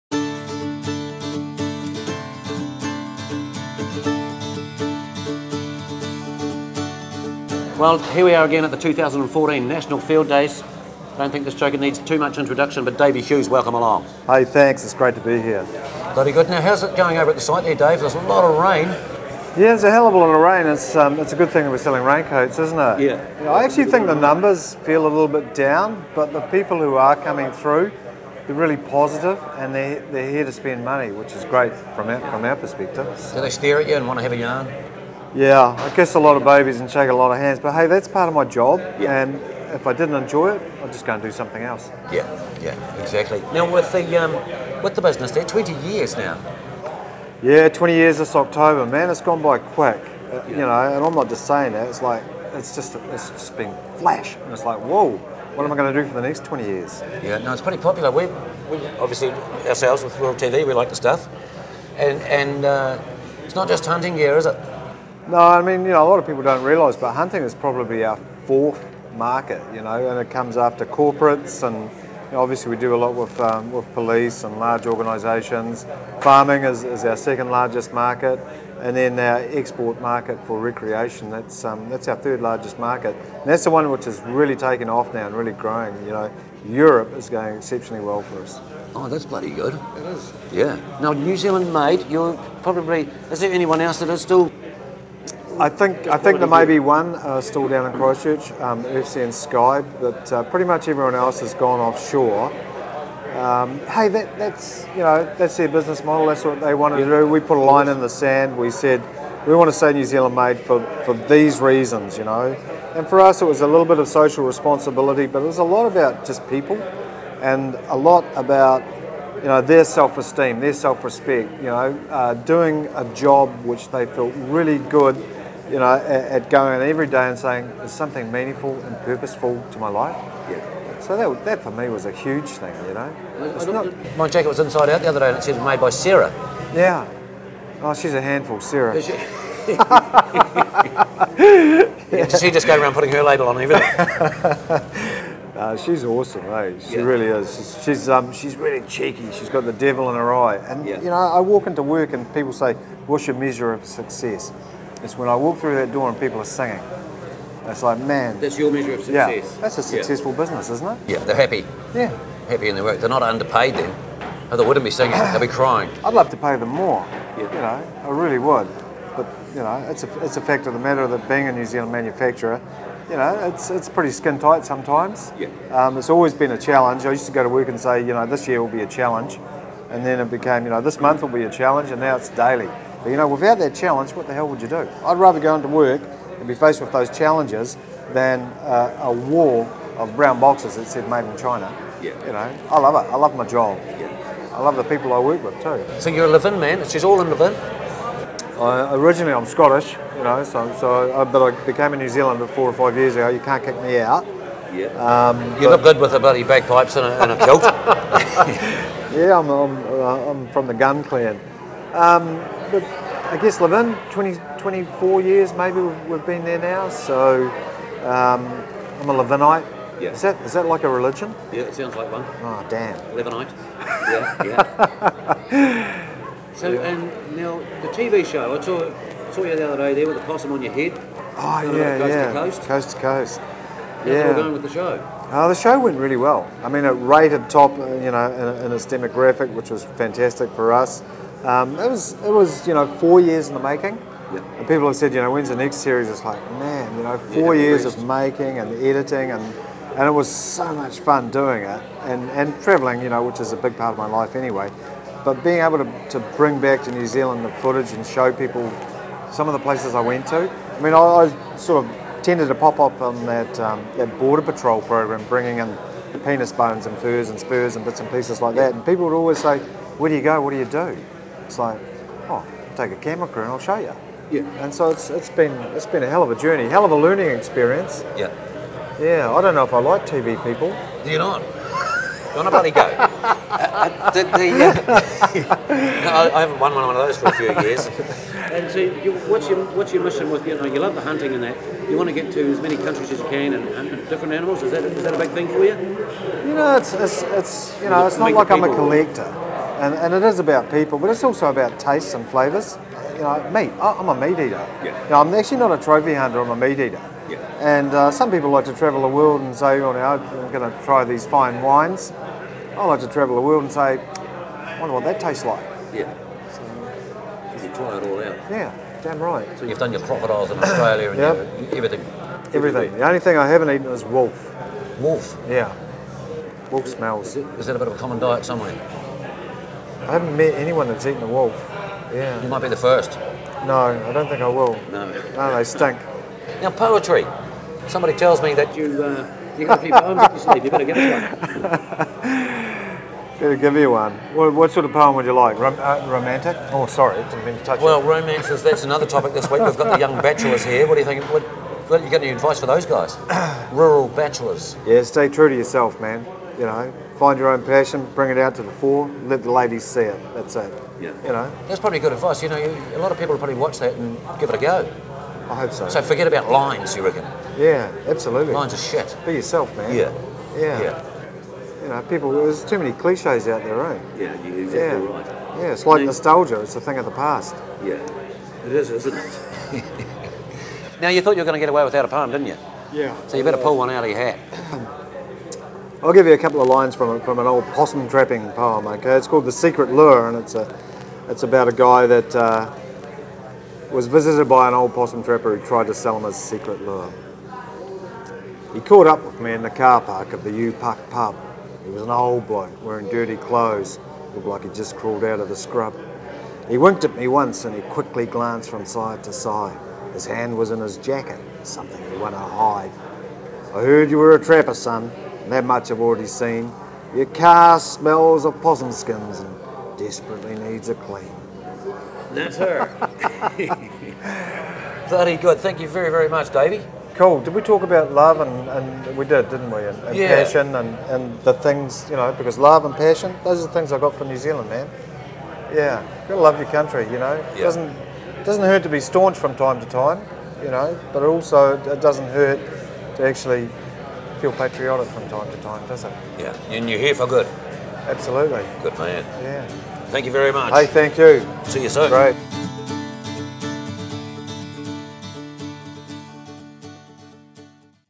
Penis Bones, Possum Traps & Passion for NZ Made - 2014 FIELDAYS
Tony Glynn was perched at his table in the on-site Rural TV studio for four days yarning to CEOs, Project Leaders & hard-case buggers to uncover new products, new innovations & cracking a few jokes.

DAVEY HUGHES - Co-Founder of SWAZI Clothing & international hunter!

Like a pair of old mates, Tony enjoys his yarn with NZ hunting & business identity Davey Hughes.

Davey enjoys Fieldays shaking hands & kissing babies among selling rain-coats. He explains that hunting, what he is well known for is only the 4th biggest market, and farming is a huge market.

Tony tries to find out if there is any business left in New Zealand still manufacturing apart from Swazi which is meaningful & purposeful to Davey even if it is skin-tight at times! Find out what Davey's measure of success is...

Davey also discusses his highly rated TV programme, Untamed, which took four years to make!

Find out why Davey travels the world hunting for meat instead of trophy and what is the one thing he hasn't eaten but would like too!

With the Rural Bachelors floating around outside of the Rural TV studio, Davey Hughes gives his romance advice to the boys as well as a wee poem!

His final words are to not be so staunch & be proud of our great country!